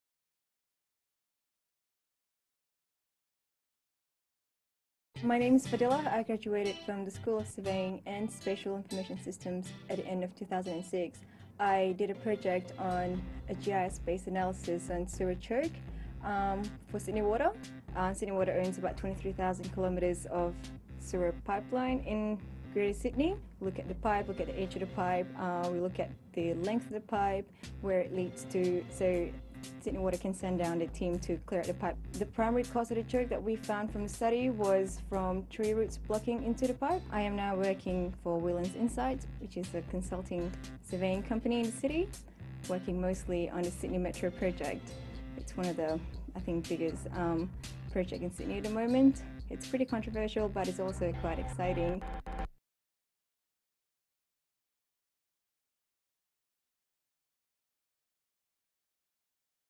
Fadillah Norzahari: Surveying and Geospatial Engineering (SAGE)
SAGE graduate Fadhillah Norzahari talks about her GIS-based analysis of sewage choke in Sydney and her new career.